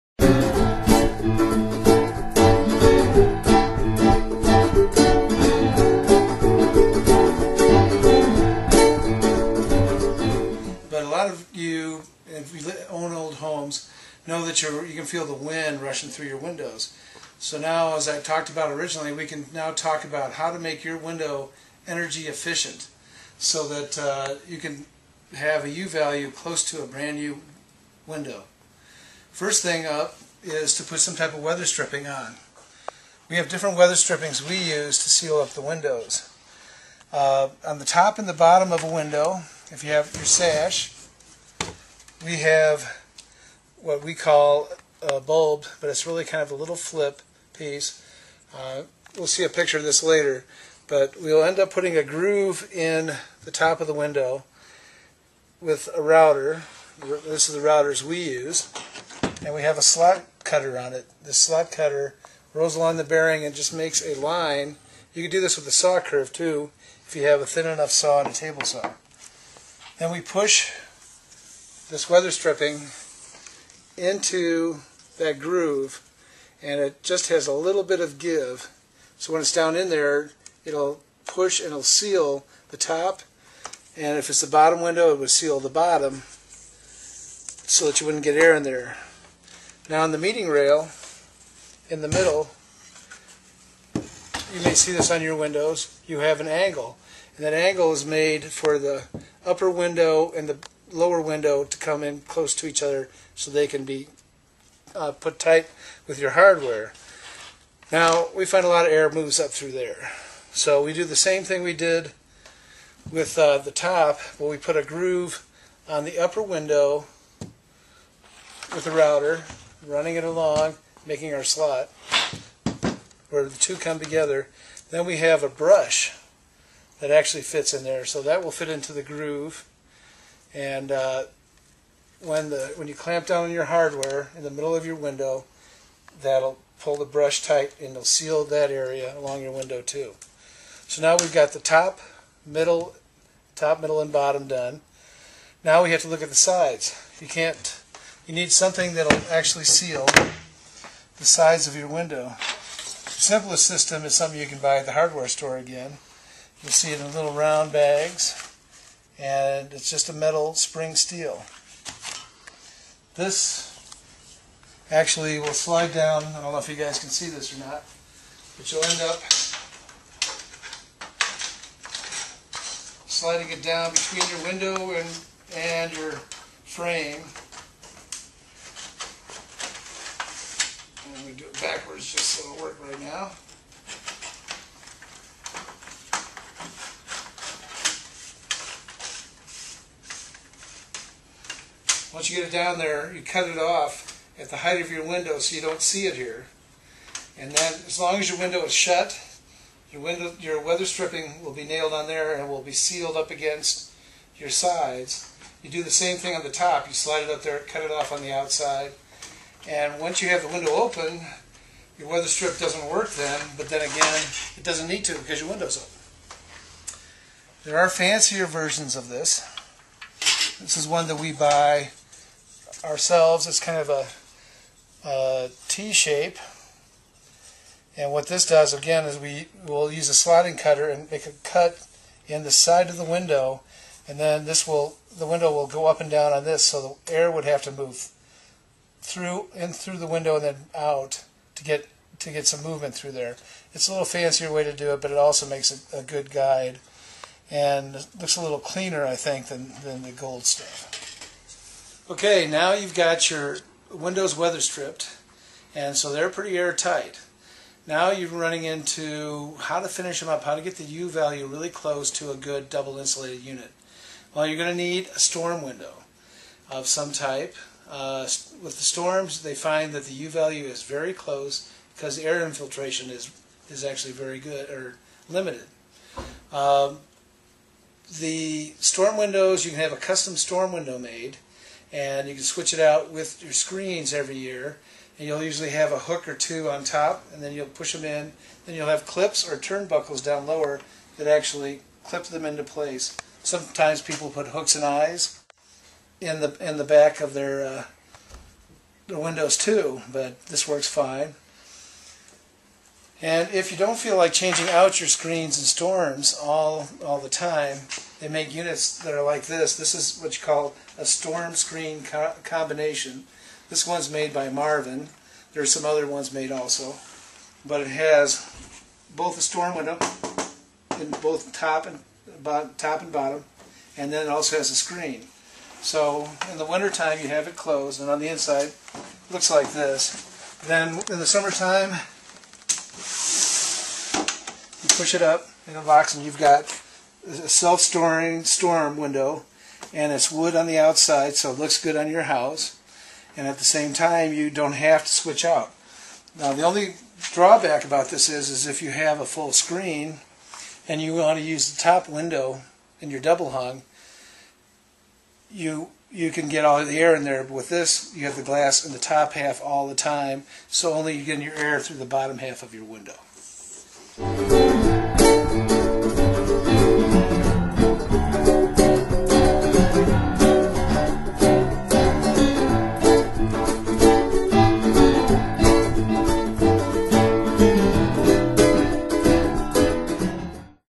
Learn to repair your windows - Trinity Glazing Edinburgh If you are a DIY en
Learn to repair your windows

If you are a DIY enthusiast and enjoy weekend projects, this video will take you through the process, step by step, of insulating old windows.
Trinity Glazing Ltd, Edinburgh
CONNECT